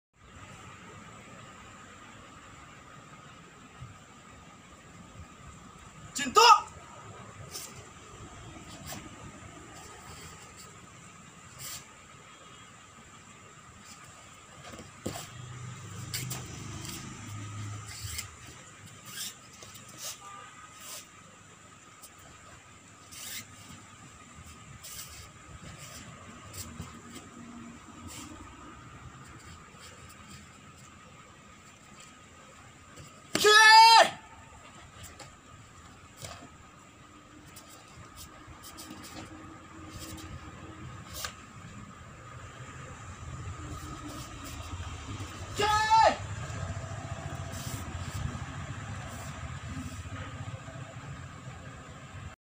SMA10801 Chinto Kata Performed by Gollo John from Itanagar, India for WKMA Championship
Karateka Gollo John from Itanagar, India performed Kata's during the Quarantine period and keep improve his skills and being the Paradigm of all karate students.

He is very much interested to support karate master's so he participate in this 2nd WKMA Internation Virtual Championship 2021 organized by World Karate Masters Association. We wish him all the best.